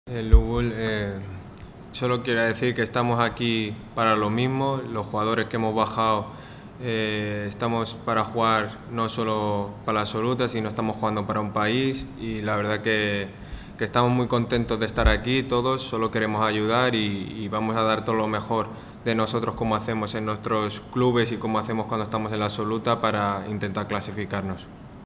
🎙| أشرف_حكيمي لاعب المنتخب الوطني لأقل من 23 سنة " نحن هنا من أجل هدف واحد لا نلعب فقط من أجل فريق
🎙| أشرف_حكيمي لاعب المنتخب الوطني لأقل من 23 سنة " نحن هنا من أجل هدف واحد لا نلعب فقط من أجل فريق ولكن من أجل بلد بأكمله, نحن سعداء للتواجد مع المنتخب الأولمبي لتقديم الدعم اللازم وكامل مجهودنا من أجل التأهل."